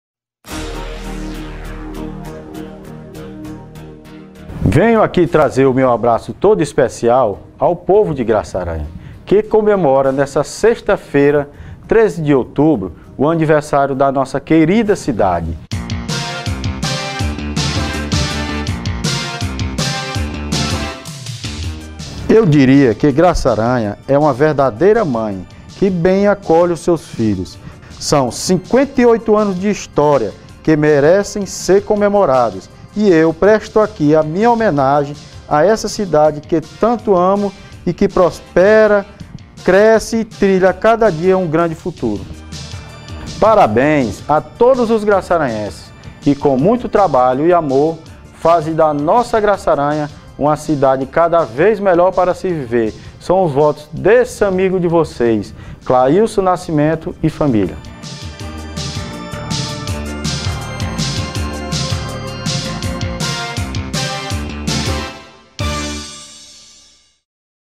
Claison Graça Aranha
Mensagem de Clailson pelo aniversario de Graça Aranha